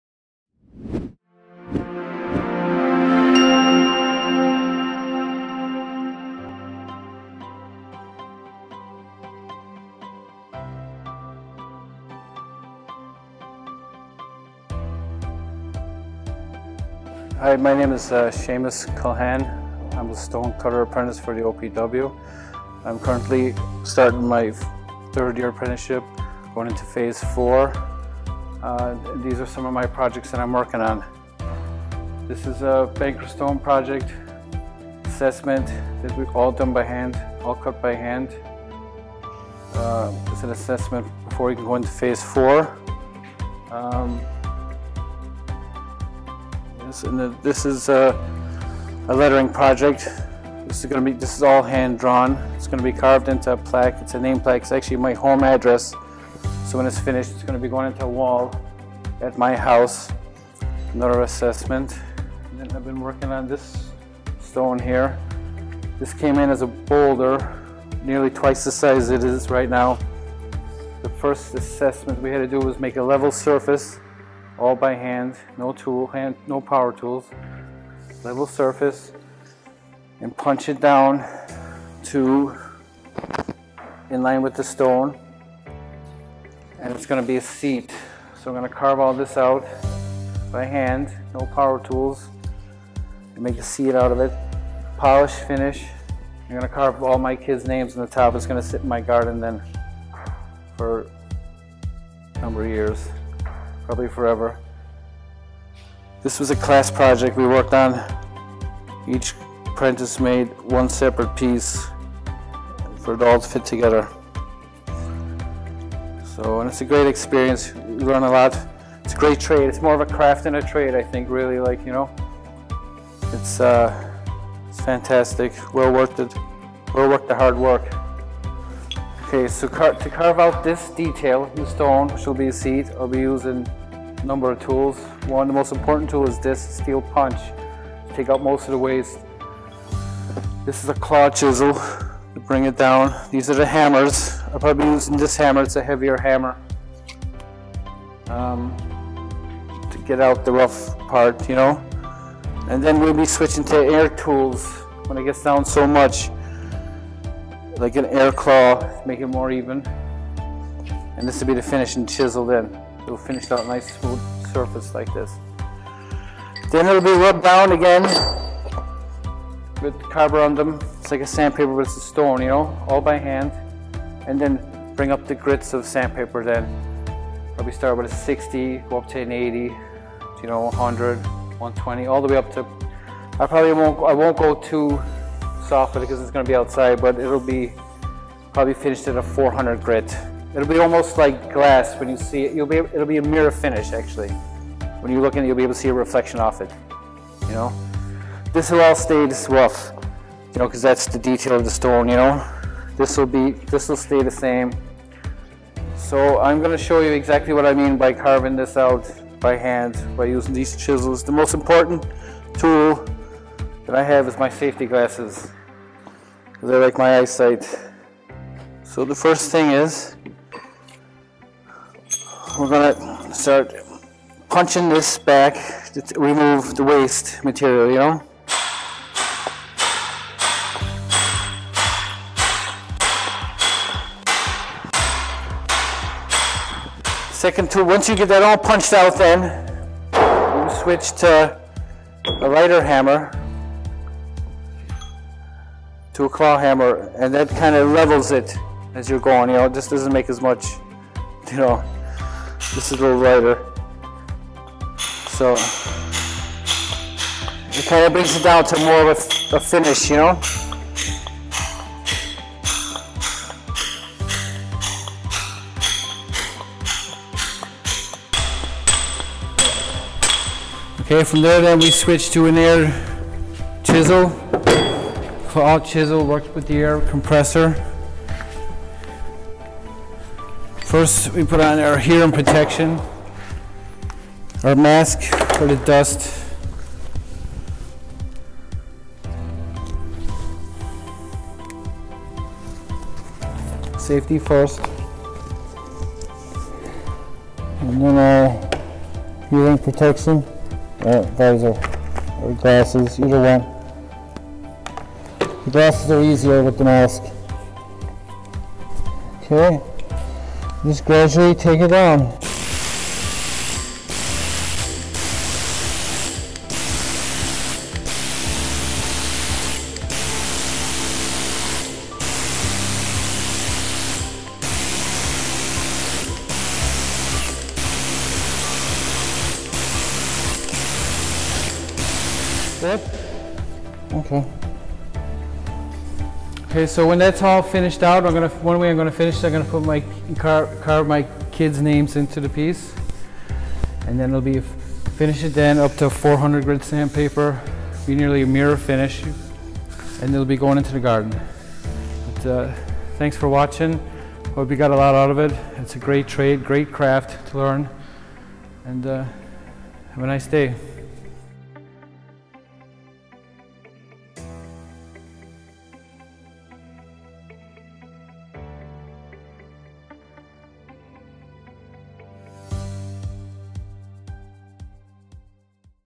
Seamus Culhane, Stonecutter-Stonemason
This video forms part of an online exhibition showcasing the remarkable skills developed by OPW apprentices and how they bring that learning to life.

to see the full exhibition.